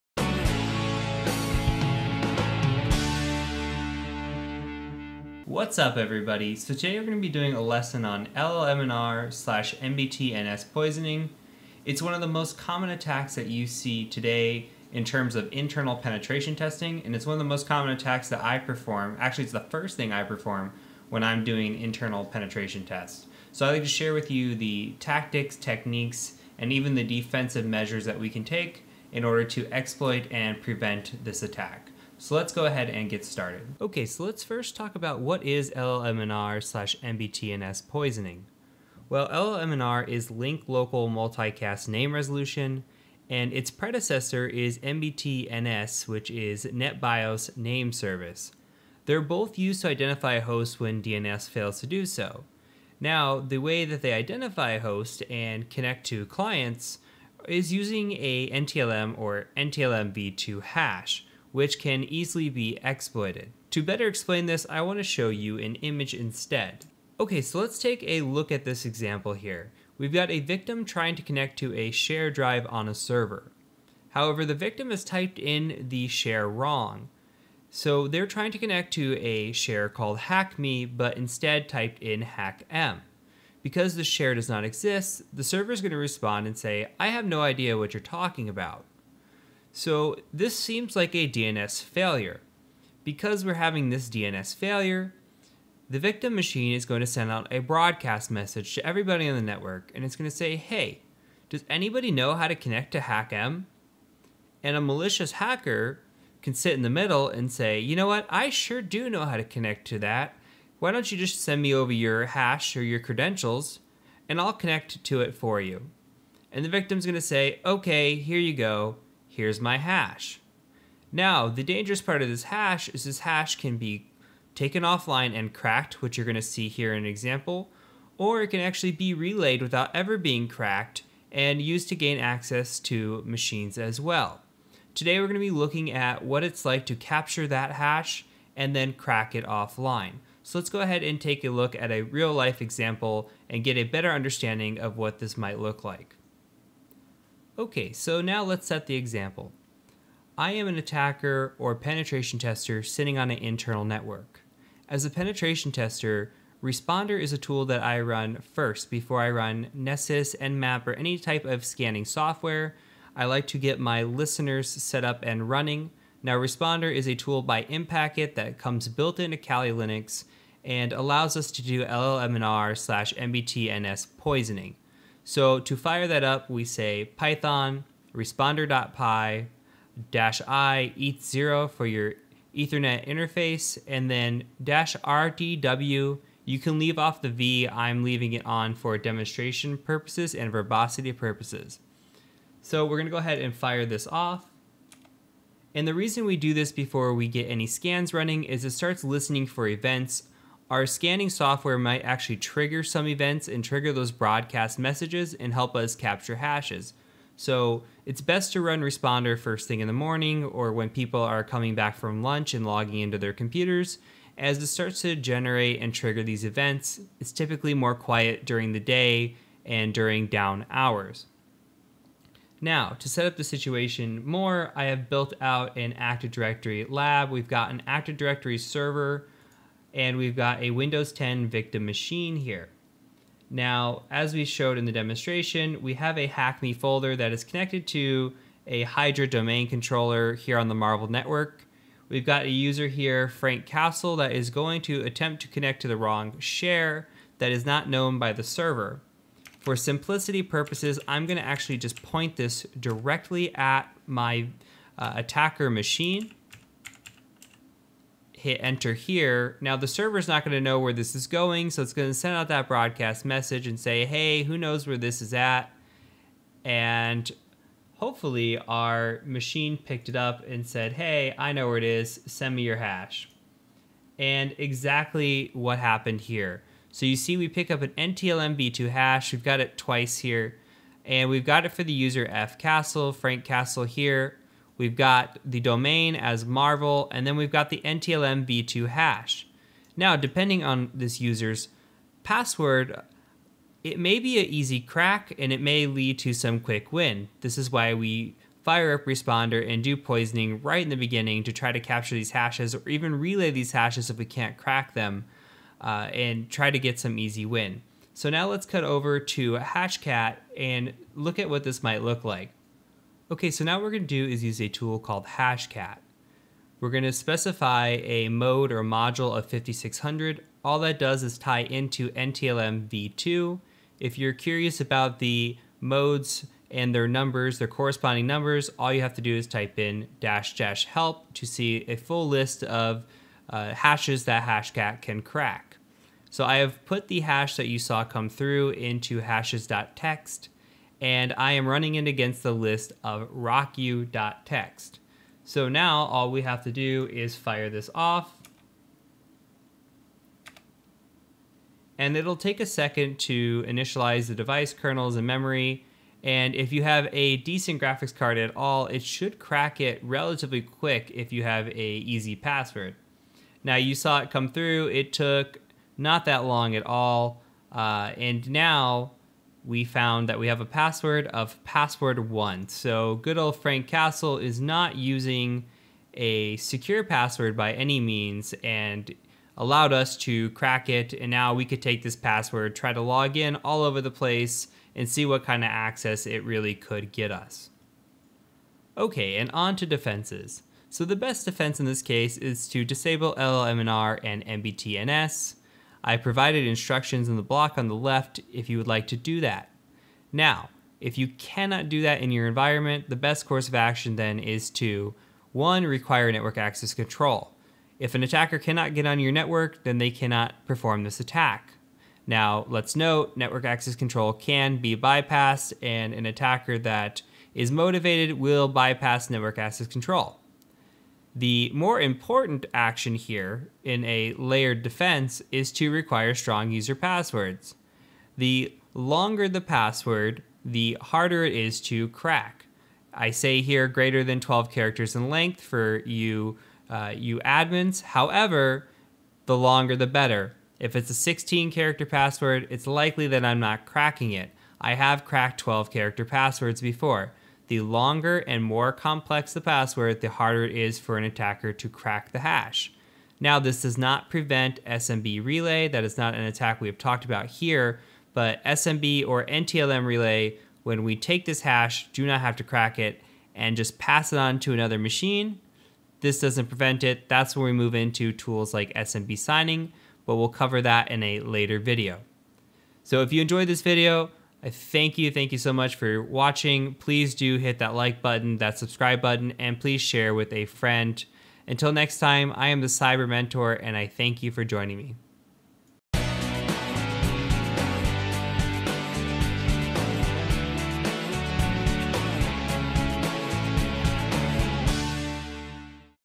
Active Directory Exploitation - LLMNR/NBT-NS Poisoning
0:00 - Introduction
0:33 - What is LLMNR/NBT-NS poisoning?
2:26 - Live attack demonstration w/ Responder
5:12 - Cracking NTLMv2 hashes w/ Hashcat
6:28 - Defenses